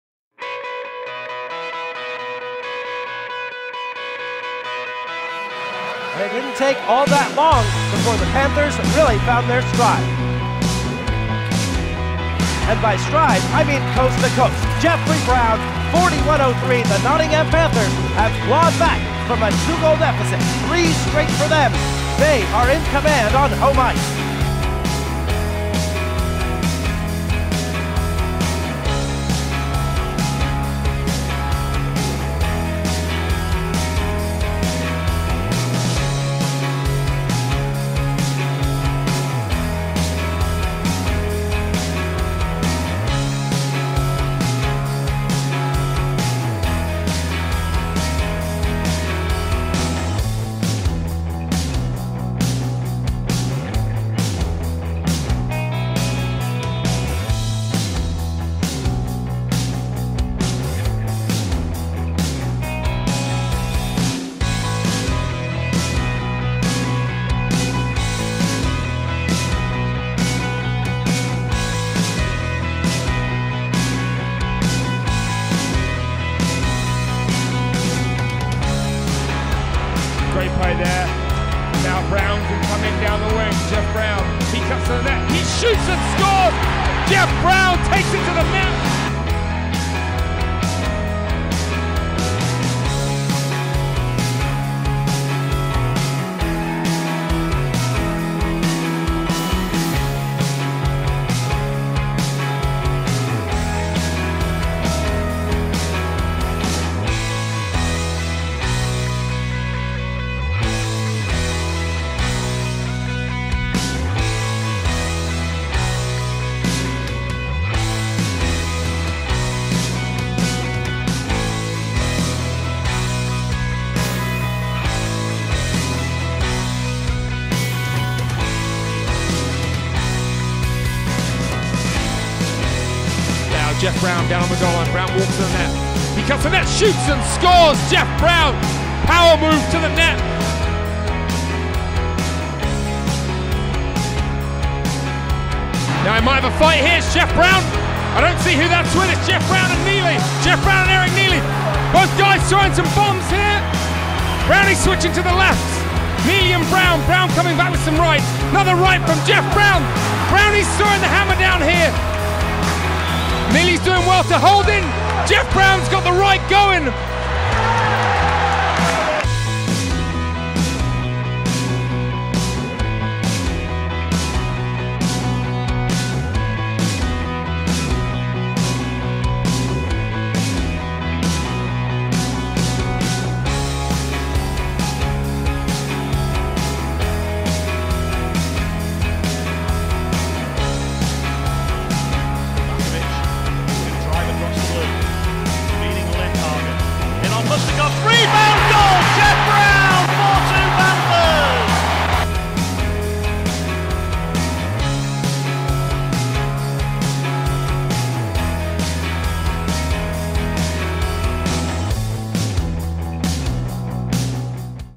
It's Lit! - Jeff Brown, Highlights 17/18
Whether it was a booming hit, highlight reel goal or good ol' fashioned scrap Jeff Brown always gave fans of The Nottingham Panthers reason to get out of their seats in 17/18!